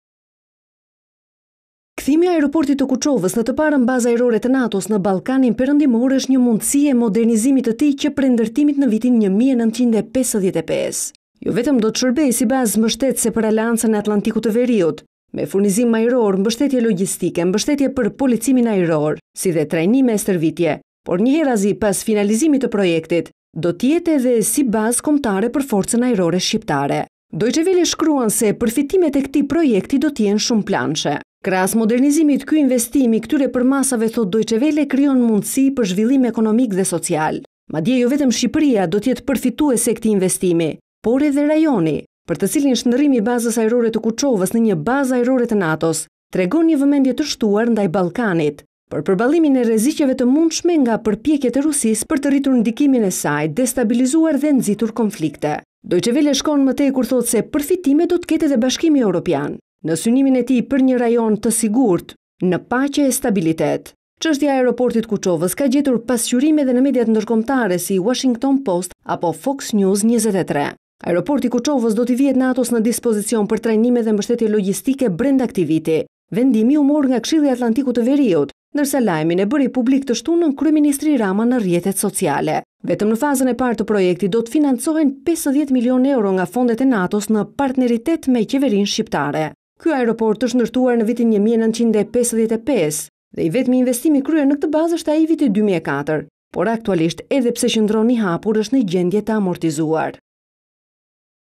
Baza e Kuçoves, perfitime shumeplaneshe
Kthimi i aeroportit të Kuçovës në të parën bazë ajrore të NATO-s në Ballkanin Përëndimor është një mundësi e modernizimit të tij që prej ndërtimit në vitin 1955. Jo vetëm do të shërbejë si bazë mbështetëse Aleancën e Atlantikut të Veriut me furnizim ajror, mbështetje logjistike, mbështetje për policimin ajror, si dhe trajnime e stërvitje, por njëherazi pas finalizimit të projektit do të jetë edhe si bazë kombëtarë për Forcen Ajrore Shqiptare.

Përfitimet e këtij projekti do të jenë shumëplanëshe. Krahas modernizimit, ky investim i këtyre përmasave, thotë DW krijon mundësi për zhvillim ekonomik dhe social. Jo vetëm Shqipëria por përfituese e këtij investimi do të jetë edhe rajoni, për të cilin shndërrimi i bazës ajrore të Kuҫovës në një bazë ajrore të NATO-s tregon një vëmendje të shtuar ndaj Ballkanit, përballlimit të rreziqeve të mundshme nga përpjekjet e Rusisë për të rritur ndikimin e saj, destabilizuar dhe nxitur konflikte.

Akoma më tej shkon DW, kur thotë se përfitime ka BE në synimin e saj për një rajon si Ballkani Perëndimor, që të jetë në siguri, paqe dhe stabilitet.

Çështja e aeroportit të Kuçovës ka gjetur pasqyrim në media ndërkombëtare si Washington Post apo Fox News 23.

Aeroporti i Kuçovës do t'i vihet NATO-s në dispozicion për trajnime dhe mbështetje logjistike brenda këtij viti. Vendimi u mor nga Këshilli i Atlantikut të Veriut, ndërsa lajmin e bëri publik të shtunën Kryeministri Rama në rrjetet sociale. Vetëm për fazën e parë të projektit do të financohen 50 milionë Euro nga fondet e NATO në partneritet me qeverinë shqiptare.

Aeroporti i Kuçovës është ndërtuar në 1955.  I vetmi investim i kryer në këtë bazë është në 2004, por aktualisht edhe pse qëndron i hapur është në gjendje të amortizuar.